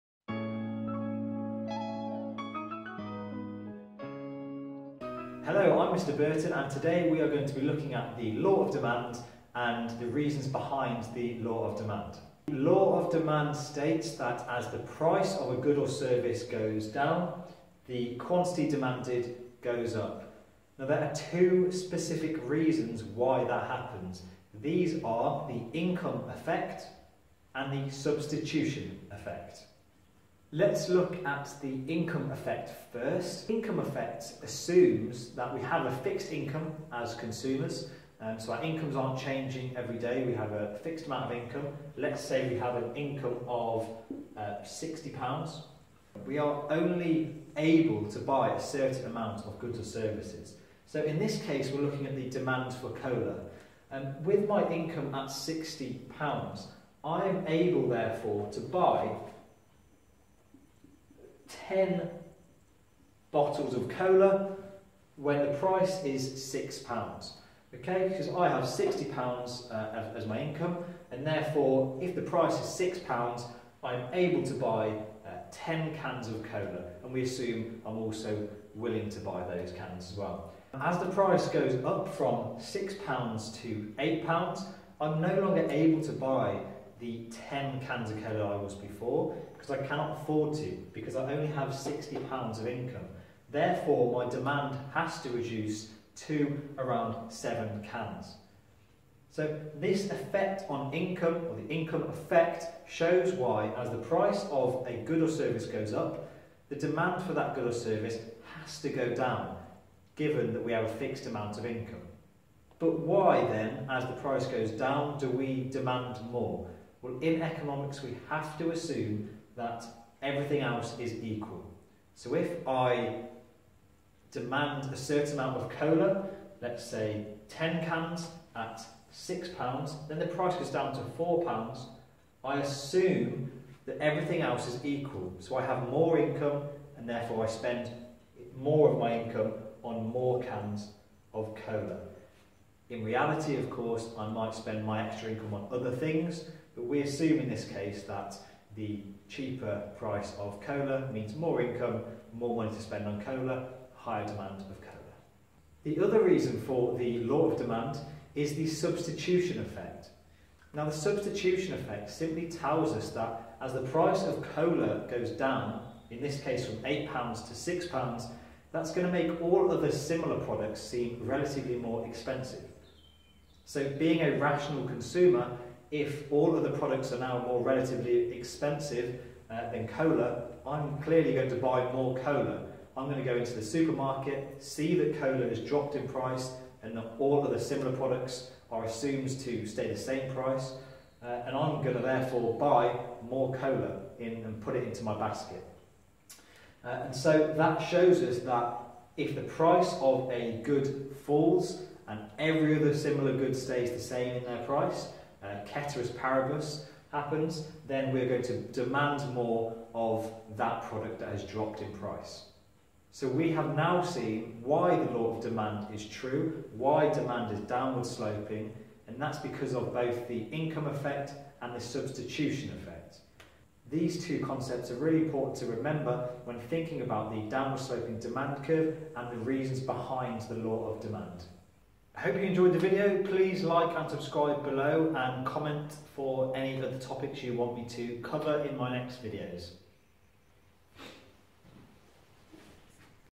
Explaining the law of demand, income and substitution effect
This is a microeconomics topic under demand theory.

This is a video explaining and illustrating the law of demand. In this video I explain the income effect and the substitution effect and how these explain why the demand curve is downward sloping.